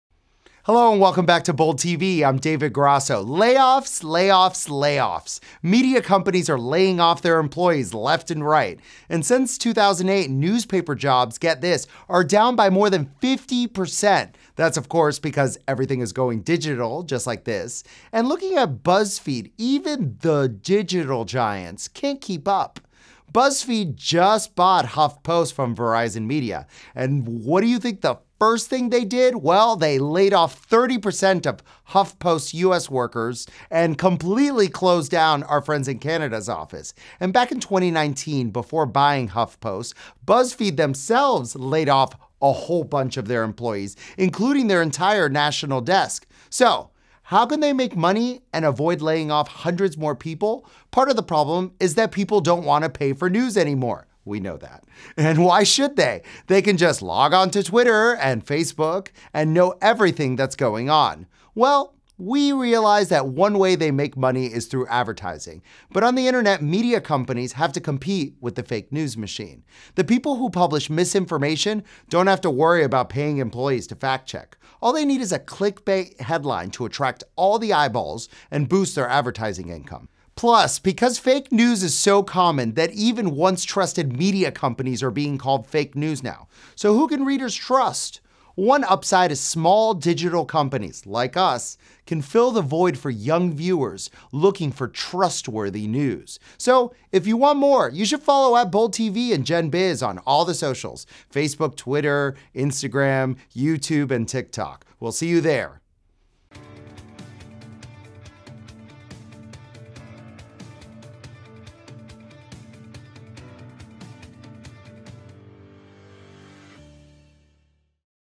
Is Print Dead AND What Is The Future Of Media?!
Layoffs, layoffs, layoffs!

Media companies are laying off their employees left and right. With everything going to digital media, newspaper jobs have gone down by more than 50% since 2008.

Even the digital giants like Buzzfeed can’t keep up. Buzzfeed just bought Huffpost from Verizon Media, and the first thing they did was lay off 30% of the Huffpost US workers, and close the Canada office. Not to mention how in 2019, before Buzzfeed bought Huffpost, they laid off many many employees including their entire national desk!

How can media companies (especially print media) make money in this day and age? We know that anyone can just log on to Twitter and Facebook, and instantly find out what they need to know.

Not only that, but digital media and print media are both completing with the fake news machine. How can these companies withstand the fake news, when those companies don't need to pay employees for fact checking? All they need is clickbait and a buzzy headline.

Who can readers trust?

One upside are the smaller independent companies that are fighting trustworthy news to be brought to the millennial and gen z generations. And that's where we come in.

SAY HI ON SOCIAL:
Instagram, Twitter, and Facebook: @boldtv